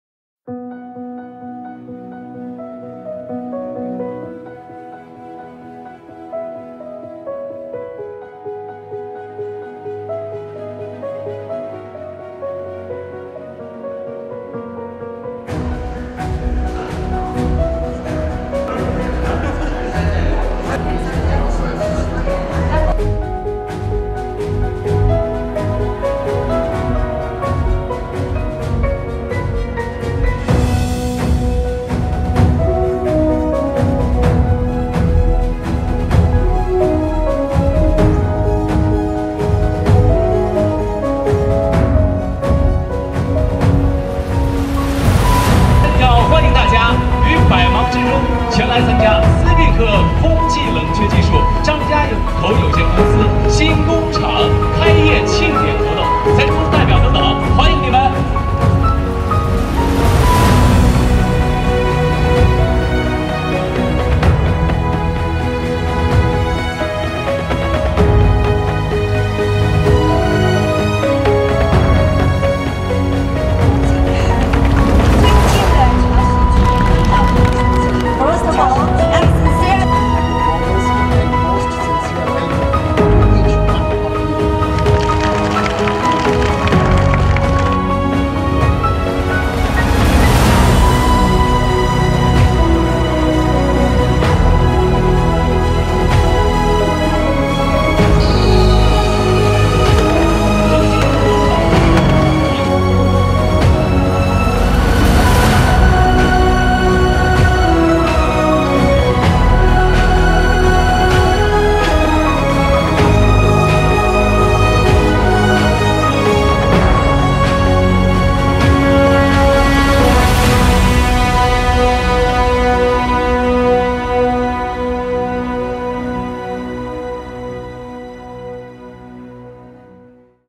New SPX Dry Cooling Manufacturing Plant in Zhangjiakou
SPX Dry Cooling is an Innovative Global Leader in Air Cooled Condensers & Coolers with the largest installed base

Last week was the grand opening ceremony of the new SPX Dry Cooling Manufacturing Plant in Zhangjiakou, China